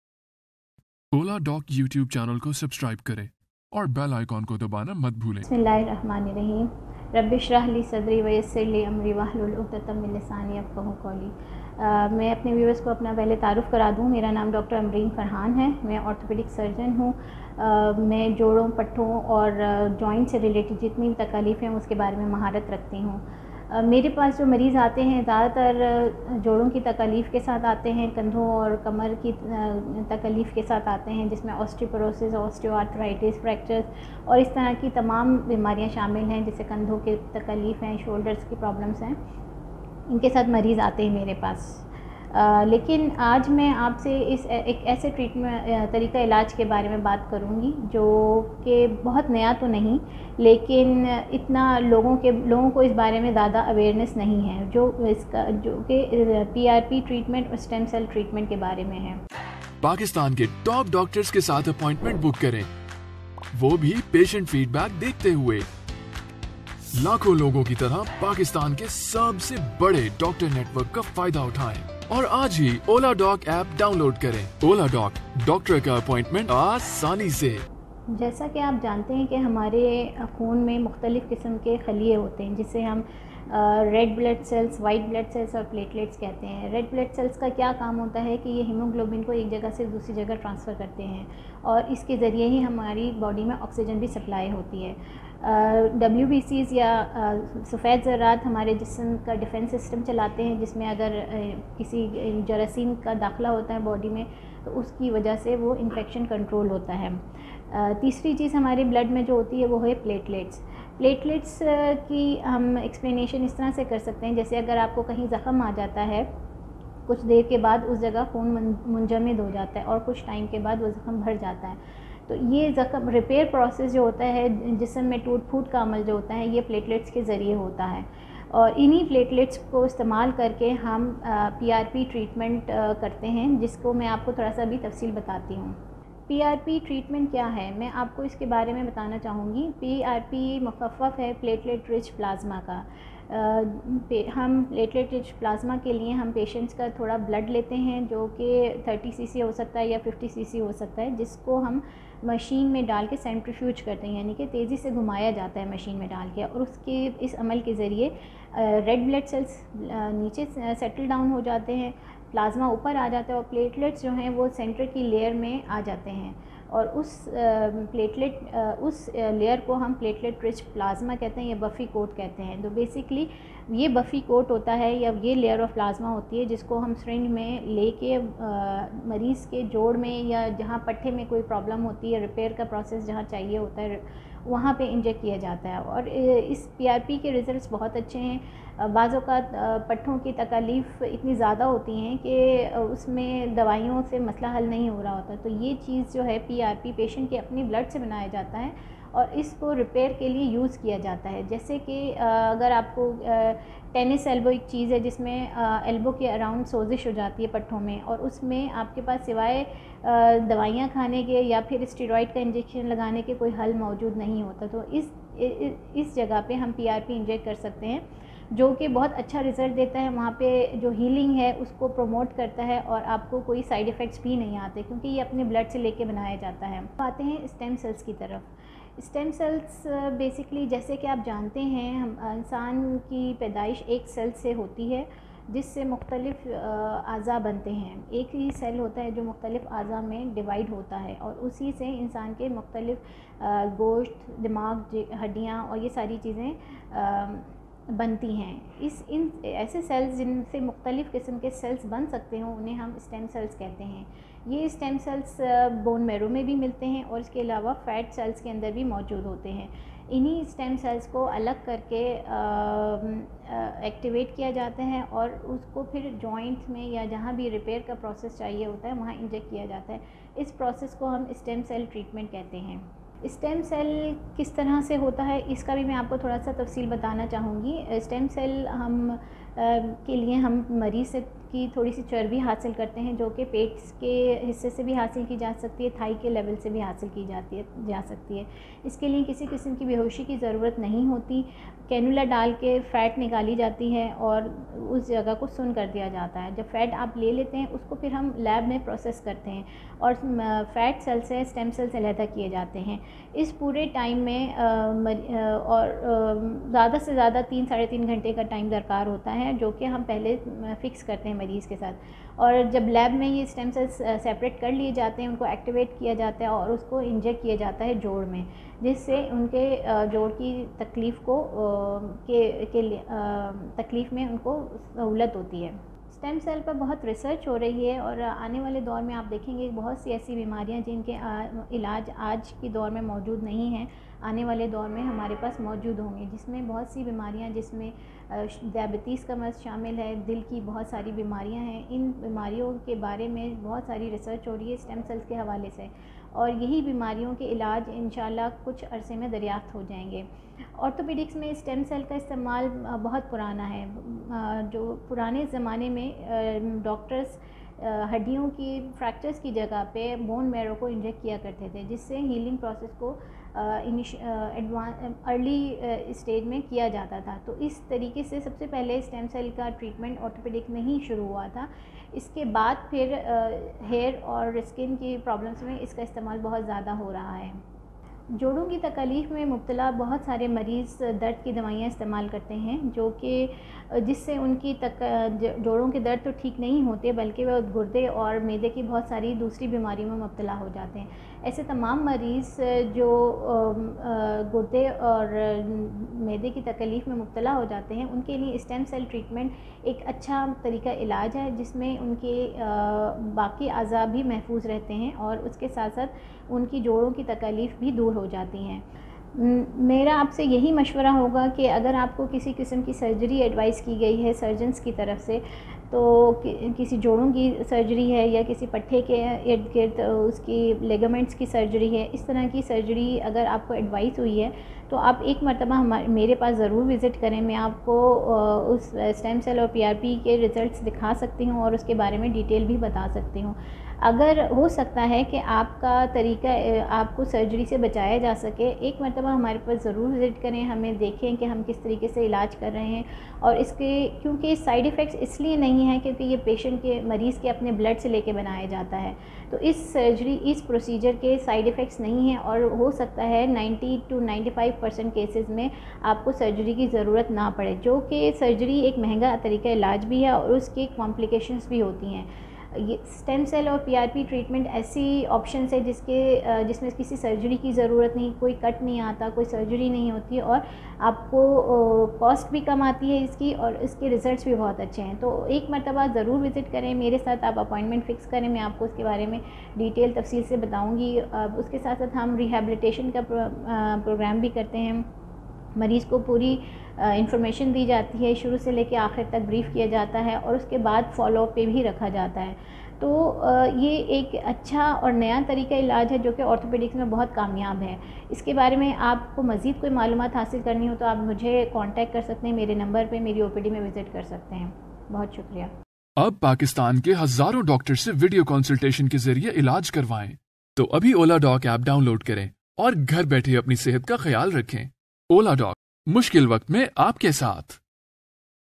Joron Pathon Ke Dard Ka Ilaj - Arthritis Joint Treatment through PRP & Stem Cell Therapy Urdu Hindi
Platelet-rich plasma (PRP) therapy uses platelets (white blood cells) inside your blood. Platelets are a type of blood cell that helps your blood clot. Platelets also help wounds heal more quickly. Stem cells are special cells inside your body that can develop into many different types of tissue. When injected into your body, stem cells may have the ability to grow into healthy new cells.

PRP and Stem Cell therapy are used to treat conditions like arthritis, ligament injuries, strains and pains, partial tears, cartilage injuries, and muscle injuries (joron aur pathon ke dard ka ilaj). These revolutionary noninvasive treatments relieve pain and promote long-lasting healing of many bone, muscle, joint, tendon, and ligament conditions.

Dr. Ambreen Farhan is one of the top Orthopedic Surgeons in Karachi.